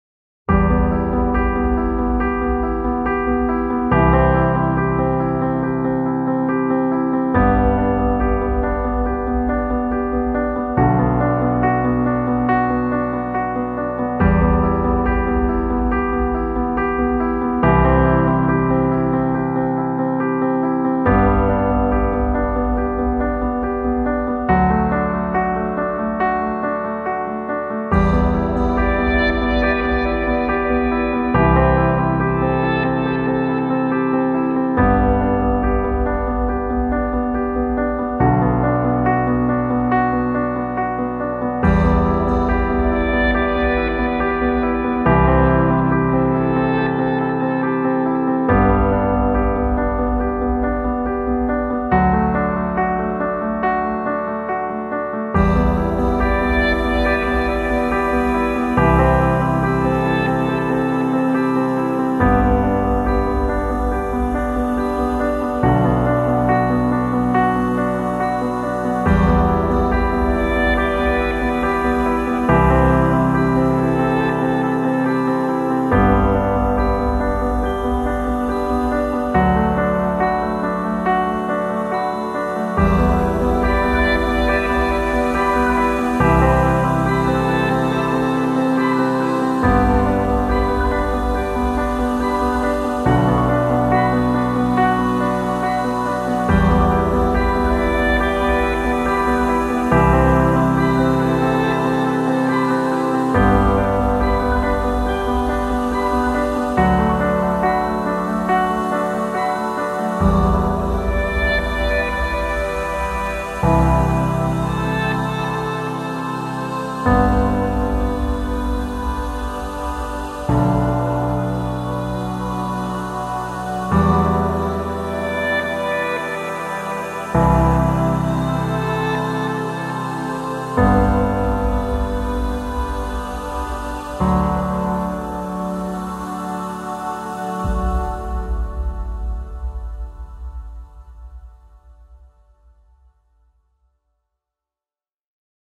IN VOLO SULLA VALSANGONE
Riprese aeree d'archivio, effettuate tra il 2013 e il 2019.